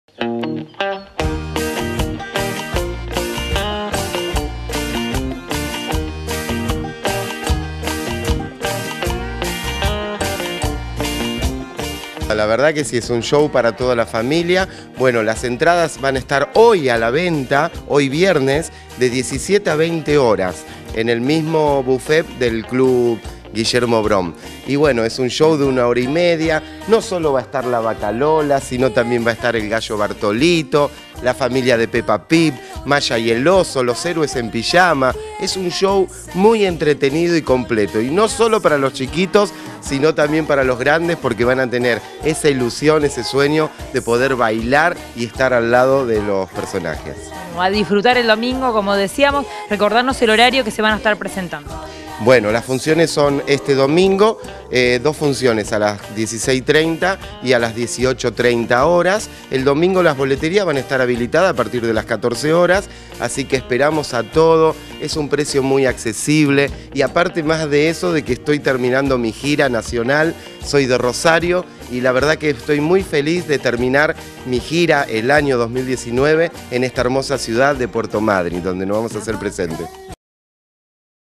Llegan a Madryn "Los Amigos de la Granja"
Funciones el domingo 16:30 y 18:30 en el Club Brown.
Hoy en venta las entradas.